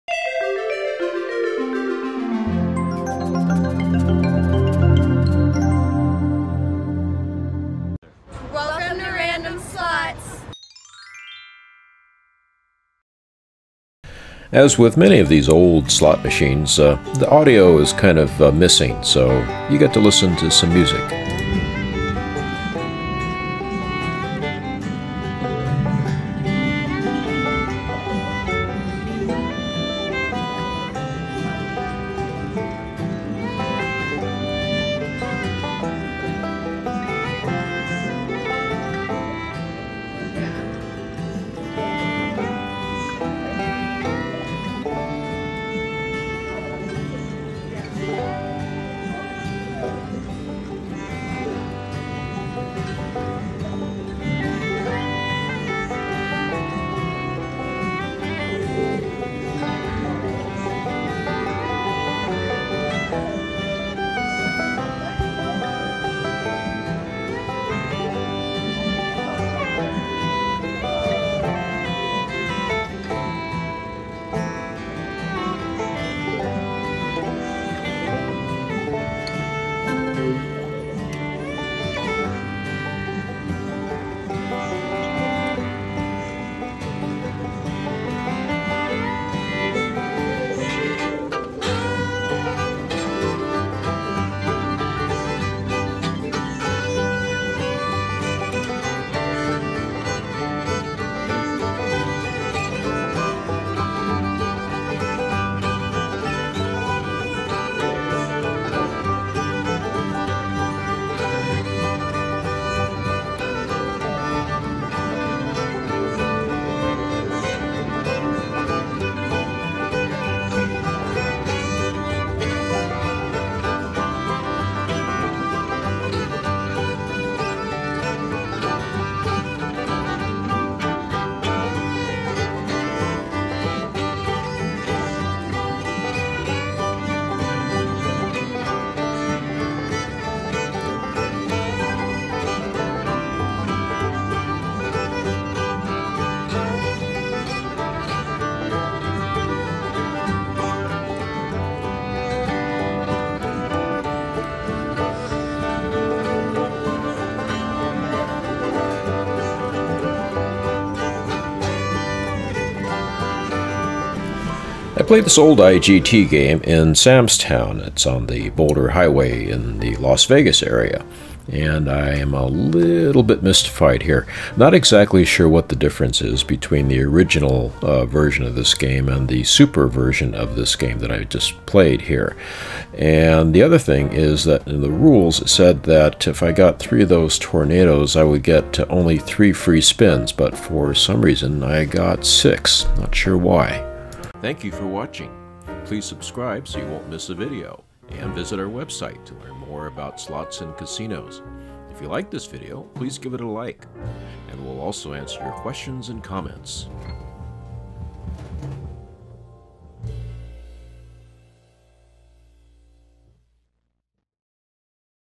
Super Money Storm Slot Machine Bonus
Casino: Sam's Town, Vegas
Slot Brand: IGT
When Played: Oct 2021